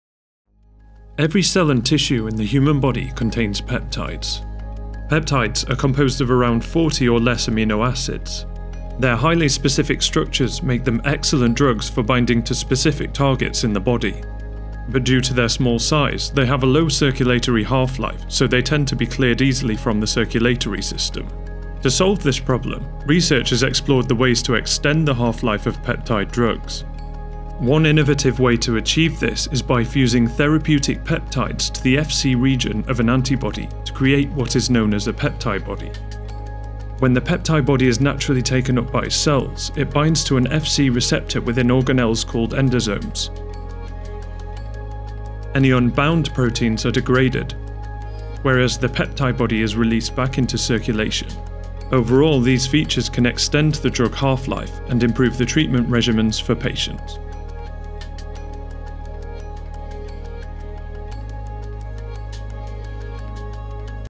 How Does It Work? | Peptibodies
Research has explored the ways to extend the half-life of peptide drugs. One innovative way to achieve this is by fusing therapeutic peptides to the Fc region of an antibody to create what is known as a peptibody.

When the peptibody is naturally taken up by cells it binds to an Fc (antibody) receptor within organelles called endosomes. Any unbound proteins are degraded, whereas the peptibody is released back into circulation.

Overall, these features can extend the drug half-life and improve the treatment regimens for patients.